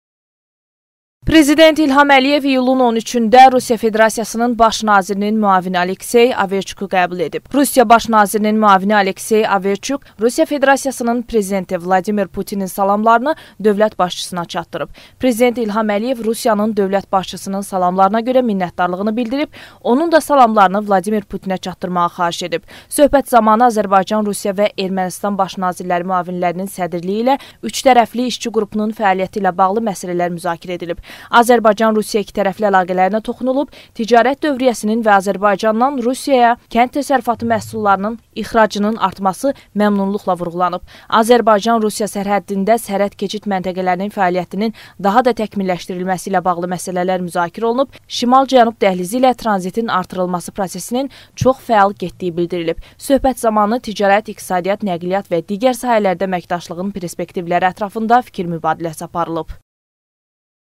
İşçi qrupunun fəaliyyəti müzakirə edildi
Prezident İlham Əliyev Rusiya Baş Nazirinin müavini A.Overçuku qəbul edib